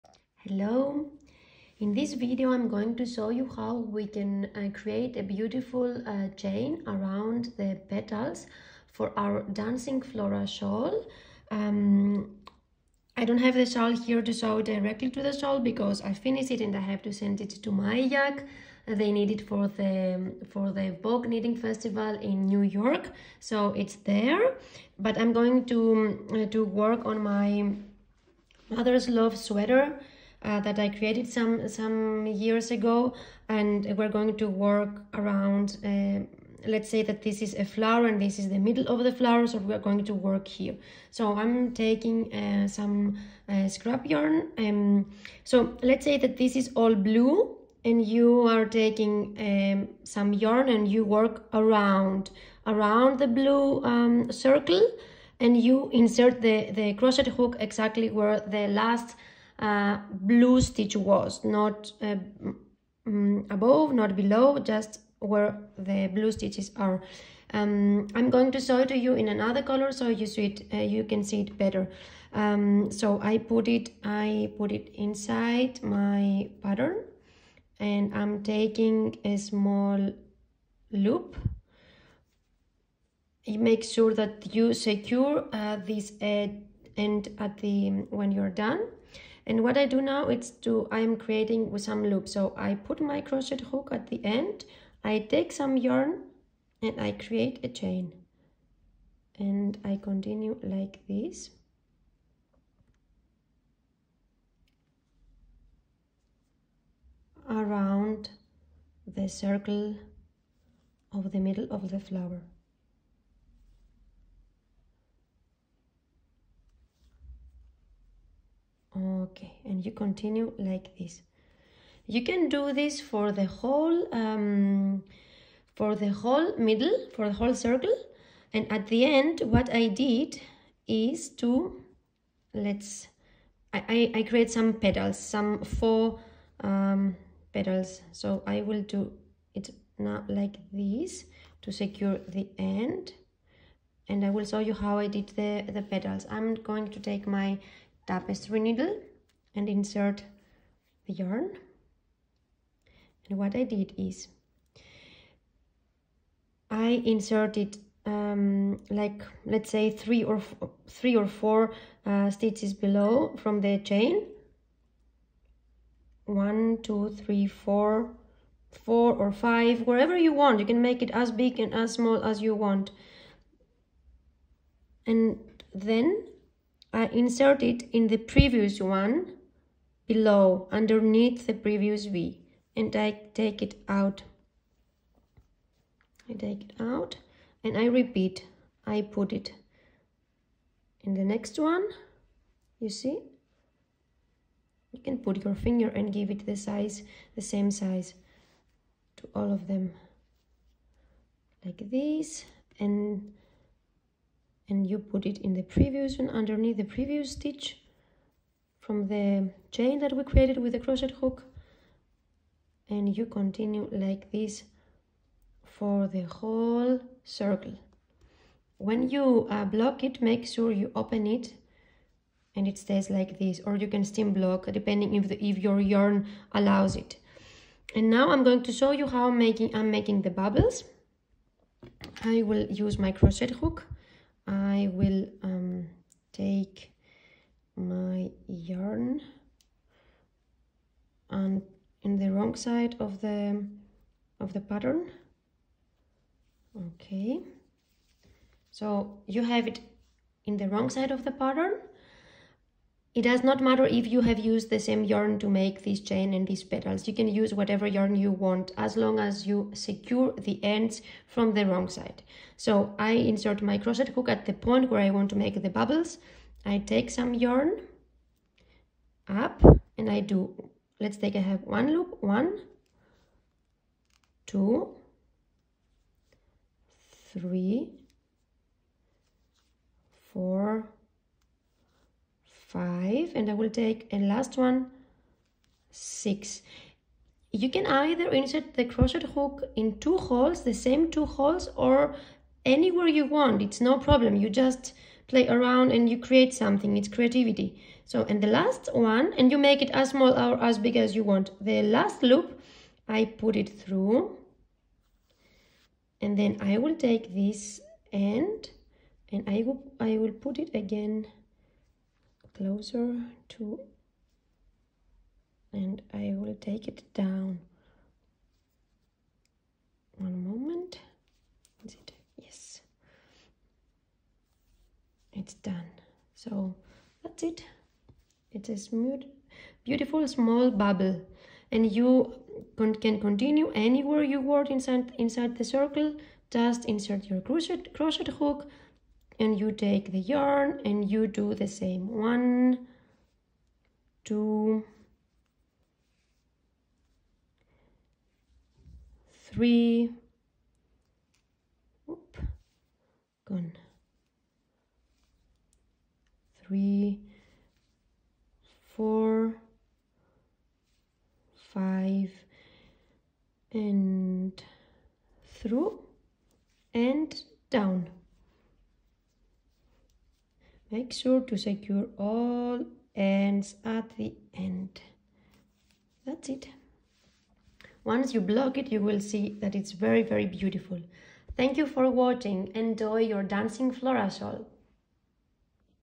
How to create a nice texture for your flowers of the Dancing Flora Shawl
Here I show you how to create a nice texture for your flowers of the Dancing Flora shawl: how to create a chain around the pistil, some faux petals and some bobbles in the middle.

You can either do it with the same or with other color. You can even do it with another quality of yarn, like mohair to make it more fun!